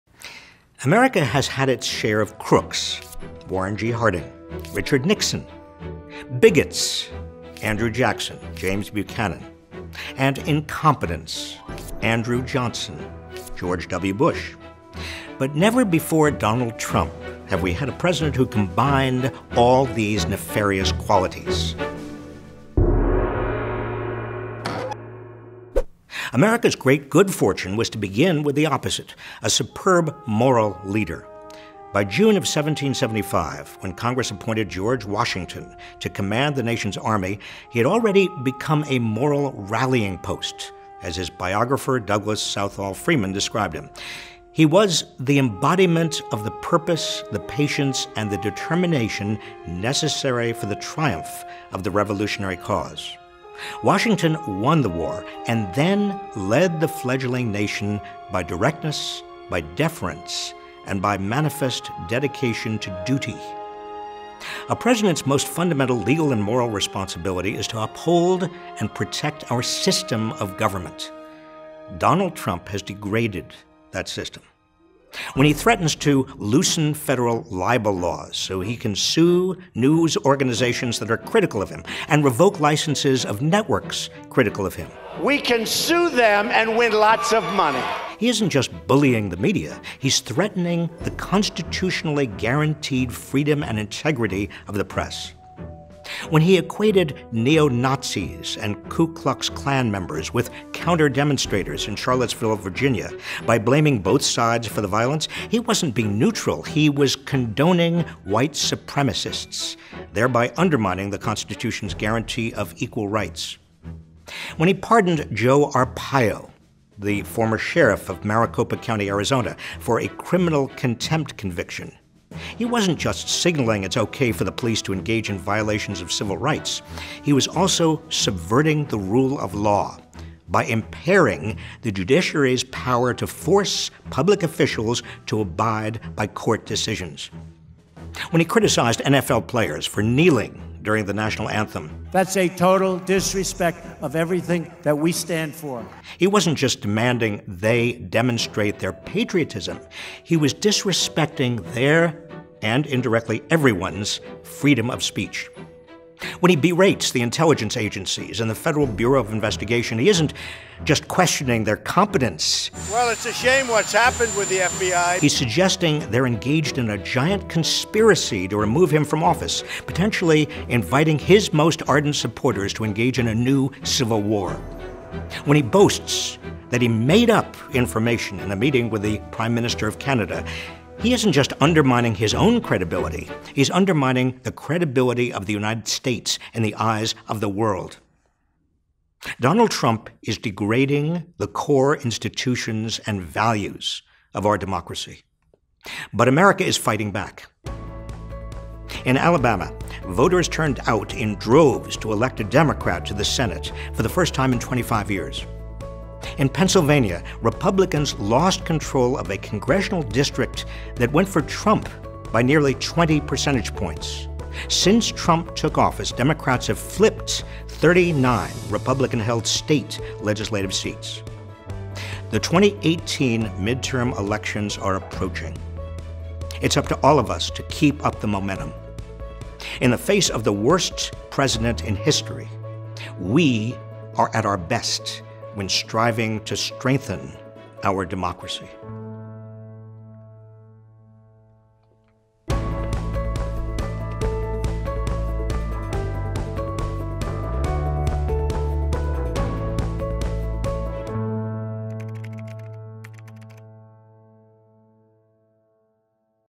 Is Trump the Worst President in History? | Robert Reich
Robert Reich compares Trump's presidency to the worst presidents in American history.